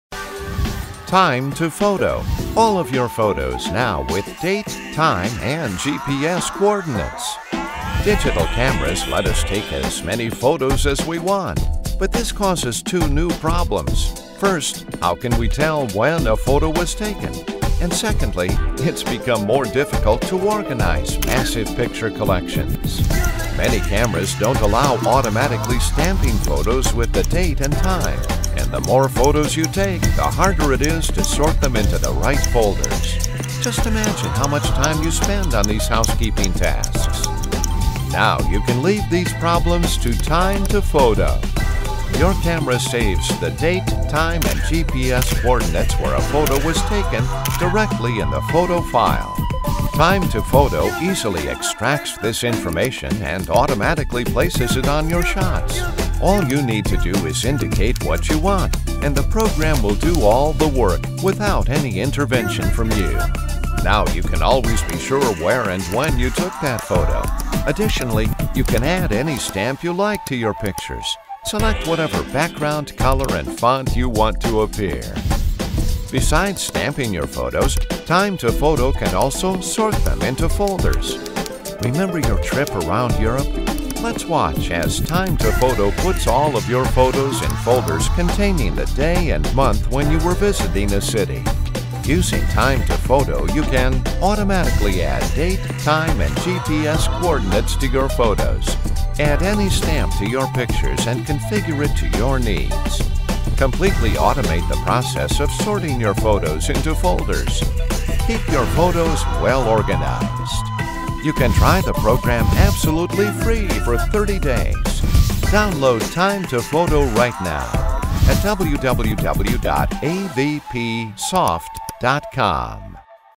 Time to Photo
TimeToPhoto: all of your photos, now with date, time and GPS coordinates!
Digital cameras let us take as many photos as we want.
But this causes two new problems. First, how can we tell when a photo was taken? And secondly, it's become more difficult to organize massive picture collections.
Many cameras don't allow automatically stamping photos with the date and time, and the more photos you take, the harder it is to sort them into the right folders.
Just imagine how much time you spend on these housekeeping tasks!
Now you can leave these problems to TimeToPhoto!
Your camera saves the date, time and GPS coordinates where a photo was taken directly in the photo file. TimeToPhoto easily extracts this information and automatically places it on your shots.
All you need to do is indicate what you want, and the program will do all the work without any intervention from you. Now you can always be sure where and when you took that photo! Additionally, you can add any stamp you like to your pictures. Select whatever background, color and font you want to appear!
Besides stamping your photos, TimeToPhoto can also sort them into folders. Remember your trip around Europe? Let's watch as TimeToPhoto puts all of your photos in folders containing the day and month when you were visiting a city.
Using TimeToPhoto you can:
automatically add date, time and GPS coordinates to your photos.
Add any stamp to your pictures and configure it to your needs.
Completely automate the process of sorting your photos into folders.
Keep your photos well-organized.
You can try the program for absolutely free for 30 days. Download TimeToPhoto right now at avpsoft.com!